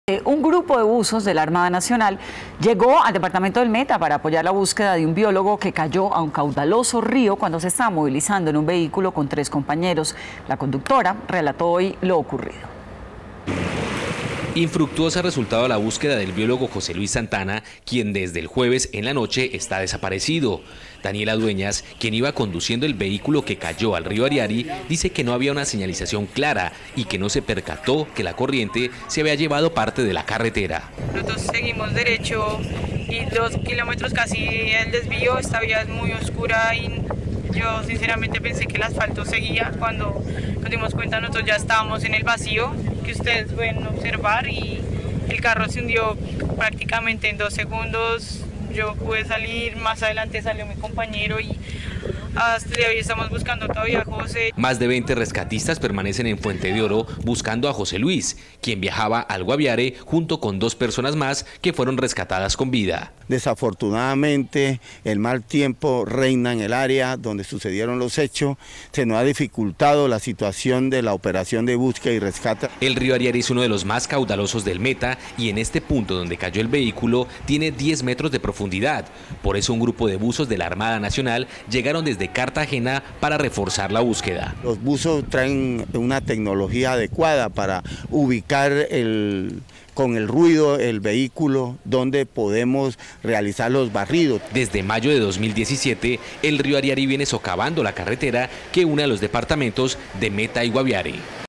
Ocupante del carro en que iba José Luis Santana narra el accidente | Noticias Caracol
Desde el jueves en la noche, el biólogo de la Universidad del Bosque está desaparecido. Se desplazaba junto con otros tres compañeros en un vehículo cuando cayeron al río Ariari.